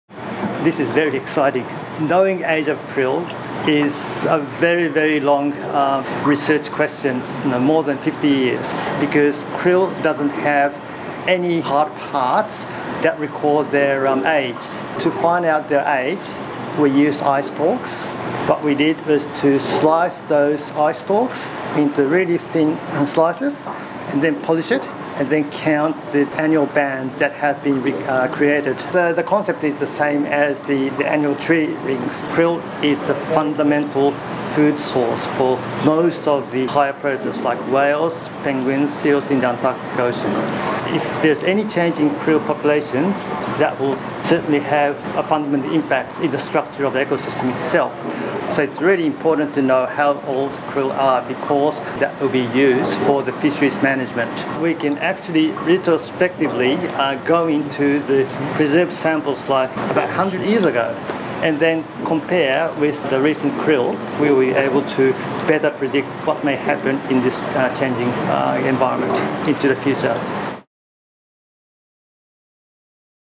Insightful approach to aging Antarctic krill
For the first time scientists have been able to determine the age of Antarctic krill by counting the growth bands in their eyestalks.